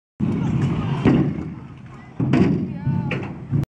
Harrison pegg: Freeze frame
Harrison pegg with a peng tuck no over fleet spine